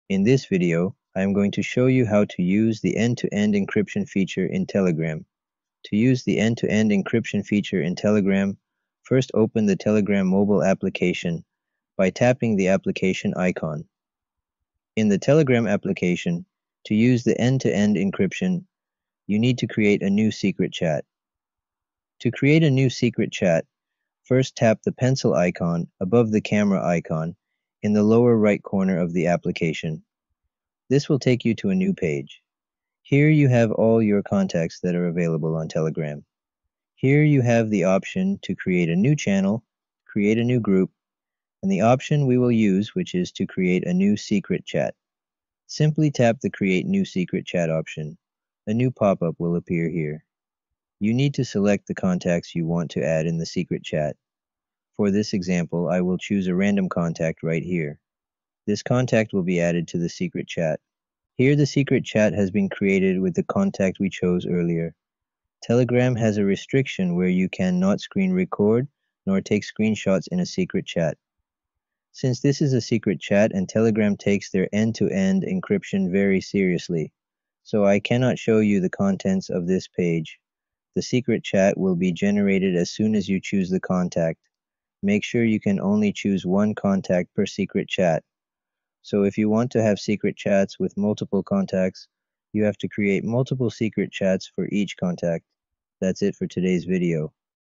How to Use End-to-End Encryption in Telegram (2024) - Enable/Disable Private Chats
In this tutorial, we'll guide you through the process of using end-to-end encryption in Telegram in 2024. Learn how to enable and disable private chats to ensure your messages are secure and protected. Whether you're discussing sensitive information or simply valuing your privacy, mastering this feature is essential for Telegram users.